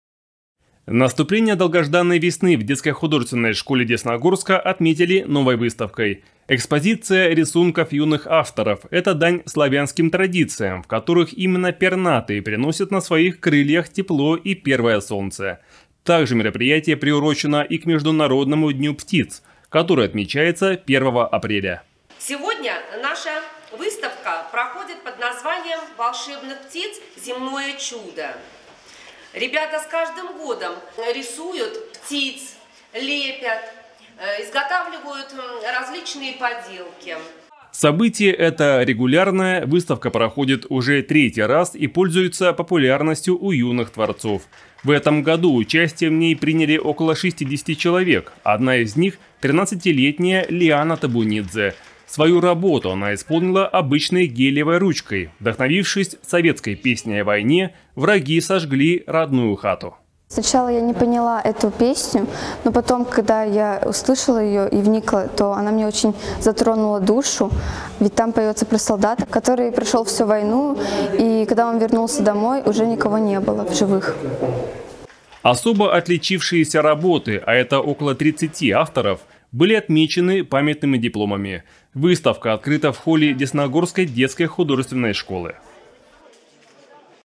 Десна-ТВ: Выставка рисунков в Десногорской детской художественной школе
Наступление долгожданной весны в Детской художественной школе Десногорска отметили новой выставкой. Экспозиция рисунков юных авторов — это дань славянским традициям, в которых именно пернатые приносят на своих крыльях тепло и первое солнце. Также мероприятие приурочено и к Международному дню птиц, который отмечается 1 апреля.